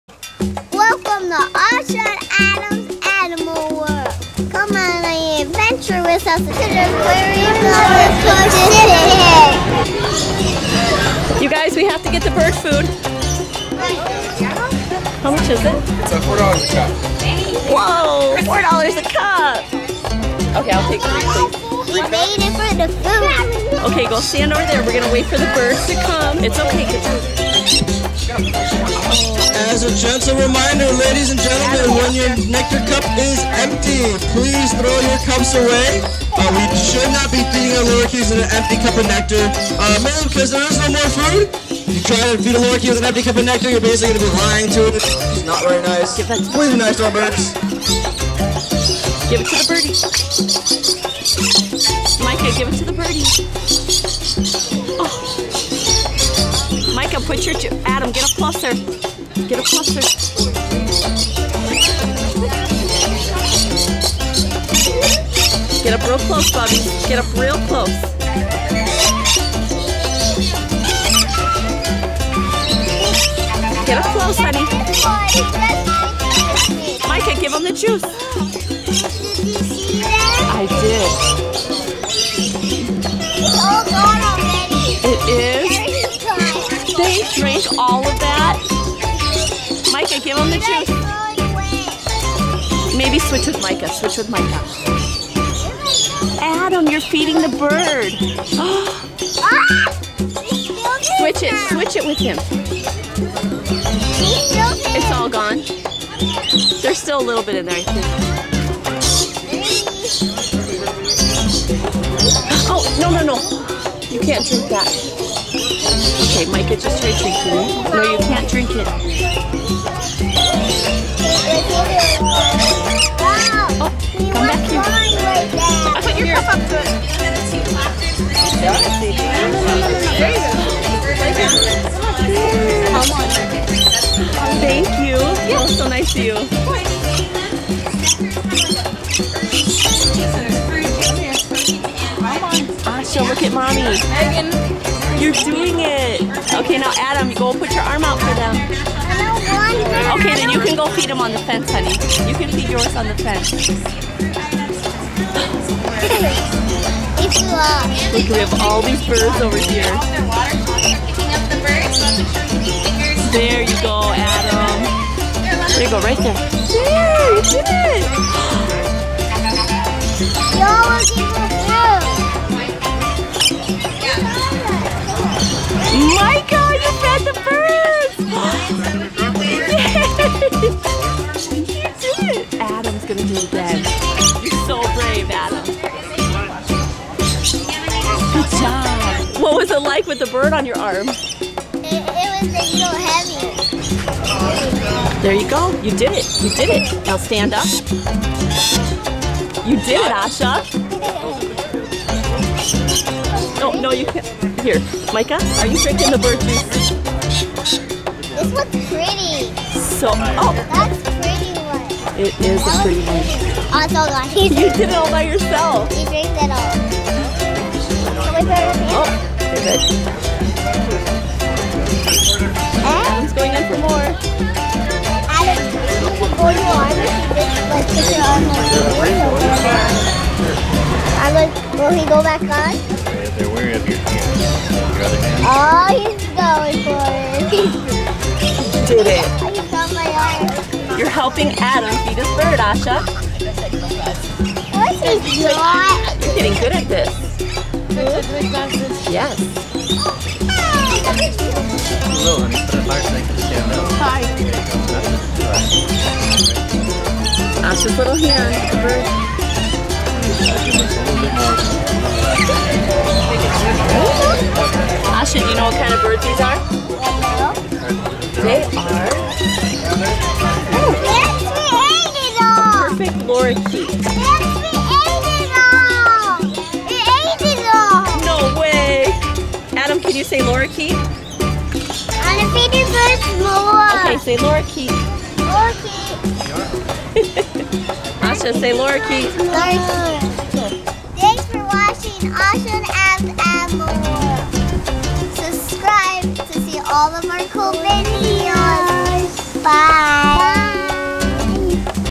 LORIKEET FEEDING Aquarium of the Pacific 2017! KIDS and Birds
Hello! Welcome to Asha & Adam's Animal World. Today we feed the Perfect Lorikeets at the Aquarium of the Pacific in Long Beach, California. They are fascinating birds with cool tongues that they use to drink sweet nectar from our hands.